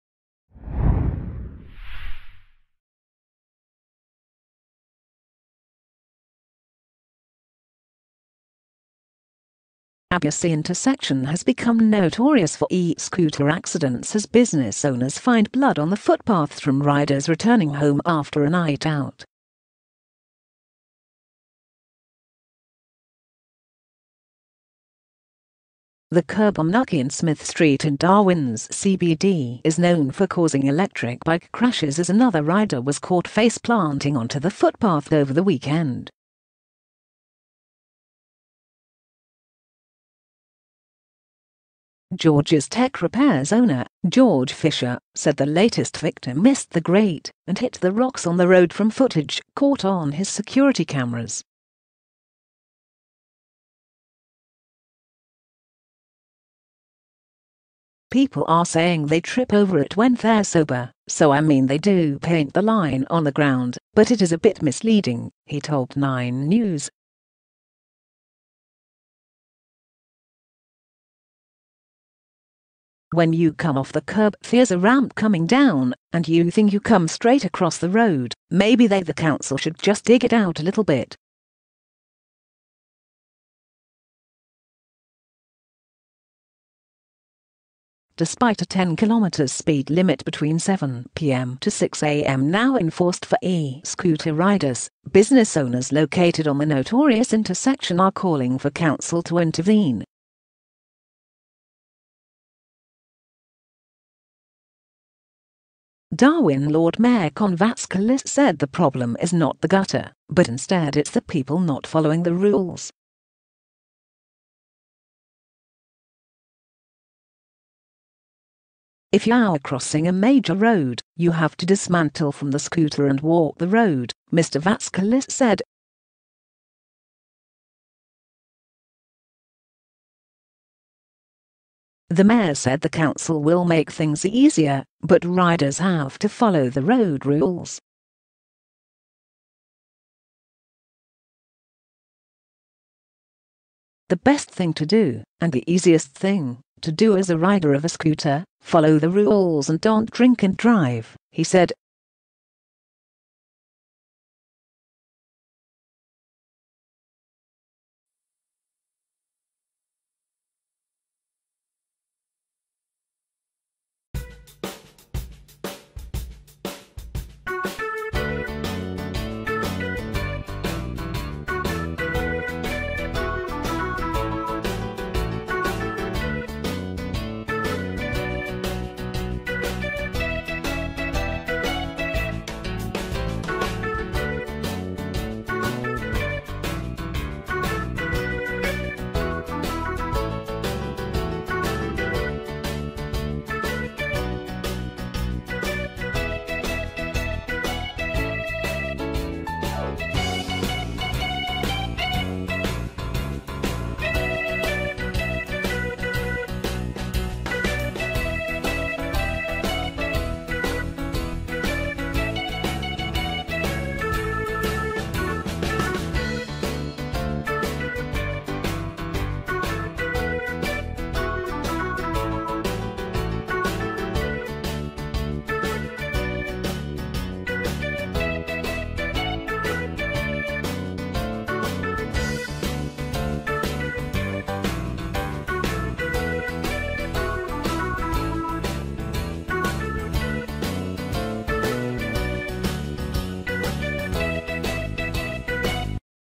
Knucky and Smith street in Darwin known for accidents after e-scooter riders fall onto gutter
Abusy intersection has become notorious for e-scooter accidents as business owners find blood on the footpath from riders returning home after a night out.The curb on Knucky and Smith street in Darwin's CBD is known for causing electric bike crashes as another rider was caught face planting onto the footpath over the weekend.George's Tech Repairs owner, George Fisher, said the latest victim missed the grate and hit the rocks on the road from footage caught on his security cameras.'People are saying they trip over it when they're sober so I mean they do paint the line on the ground but it is a bit misleading,' he told 9News.'When you come off the curb there's a ramp coming down and you think you come straight across the road, maybe they [the council] should just dig it out a little bit.' Despite a 10km speed limit between 7pm to 6am now enforced for e-scooter riders, business owners located on the notorious intersection are calling for council to intervene.Darwin Lord Mayor Kon Vatskalis said the problem is not the gutter but instead it's the 'people not following the rules'.'If you're crossing a major road, you have to dismantle from the scooter and walk the road,' Mr Vatskalis said.The mayor said the council will 'make things easier' but riders have to follow the road rules.'The best thing to do and the easiest thing to do as a rider of a scooter - follow the rules and don't drink and drive,' he said